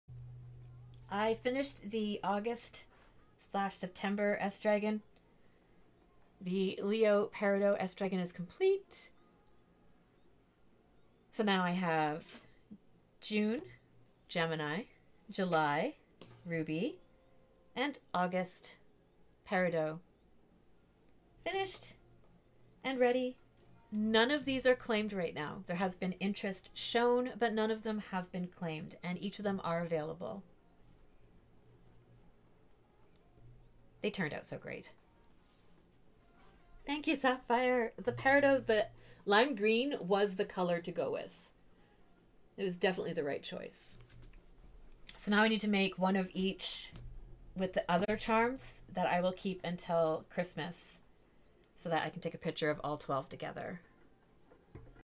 Twitch Highlight: Zodiac S-Dragons Gemini Cancer and Leo Finished
Another 3 SDragons with Zodiac inspiration are complete!

Mon-Fri 930am to 1130am MST